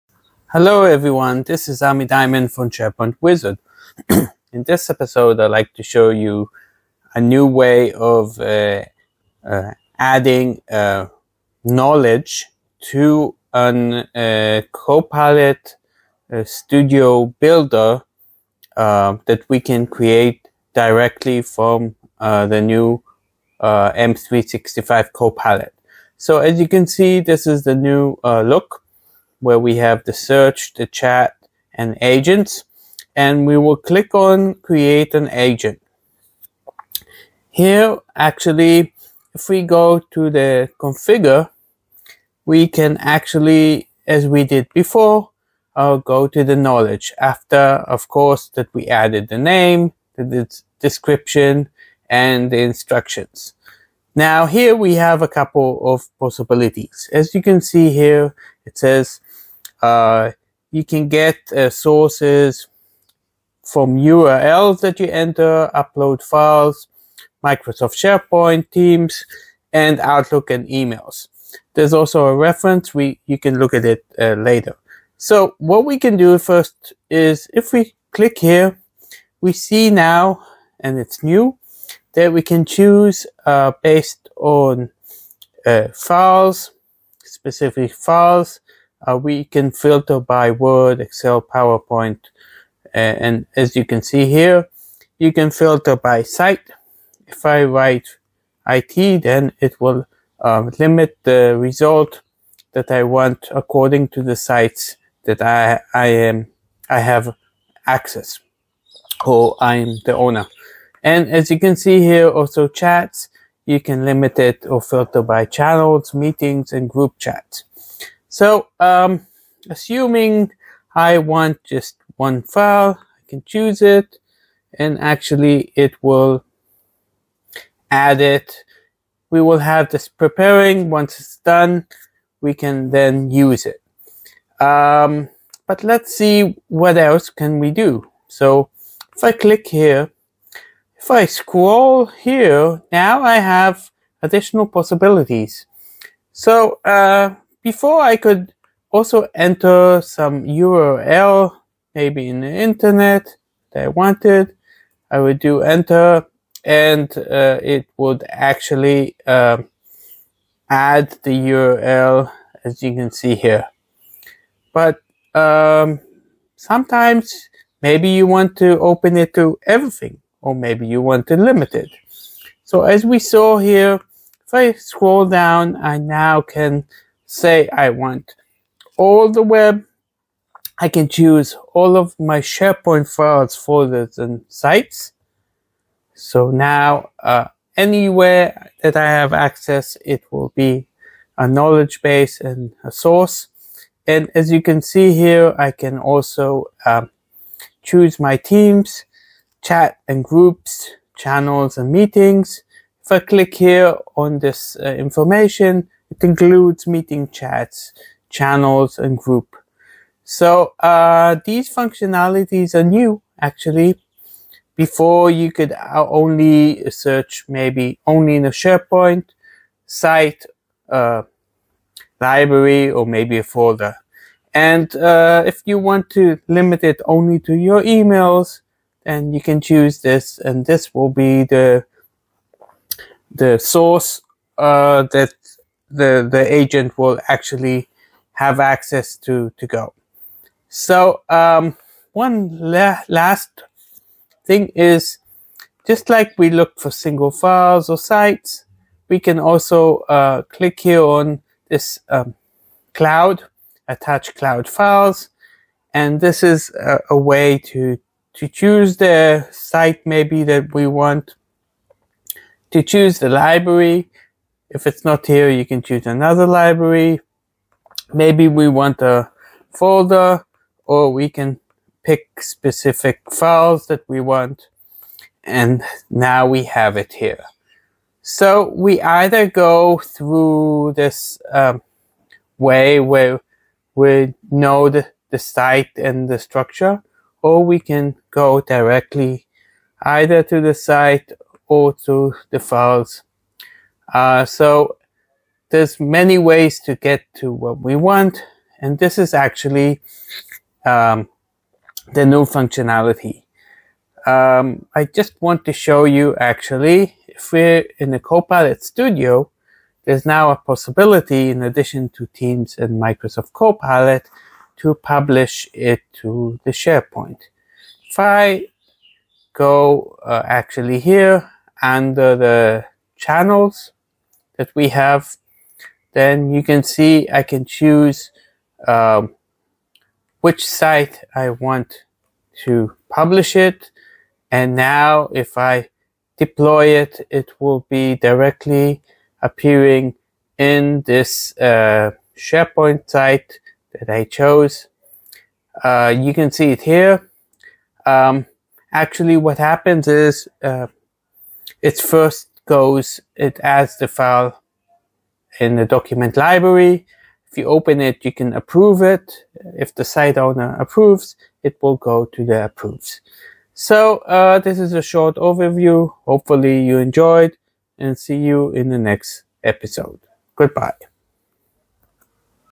Copilot Chat Agents - New knowledge sources
In this episode, I introduces a new method for adding knowledge to a Copilot Studio builder agent directly from the new Microsoft 365 Copilot interface.
Key Highlights:
• New Interface Overview: The updated layout includes sections for search, chat, and agents.
• Creating an Agent: After naming and describing the agent, users can configure its knowledge sources.
• Knowledge Source Options:
• Upload files or use URLs.
• Connect to Microsoft SharePoint, Teams, Outlook, and emails.
• Filter by file type (Word, Excel, PowerPoint), site name, or chat type (channels, meetings, group chats).
• Expanded Capabilities:
• Users can now select all SharePoint files, folders, and sites they have access to.
• Teams chats, channels, and meetings can also be included as knowledge sources.
• Email-only sources can be selected for more focused knowledge bases.
• Cloud File Attachment:
• Users can browse and attach files from specific sites, libraries, or folders.
• Publishing to SharePoint:
• Agents can now be deployed directly to a SharePoint site.
• The deployment process includes approval by the site owner before the agent becomes active.
Final Note:
This episode showcases the enhanced flexibility and integration options in Copilot Studio, making it easier to build and deploy intelligent agents across Microsoft 365.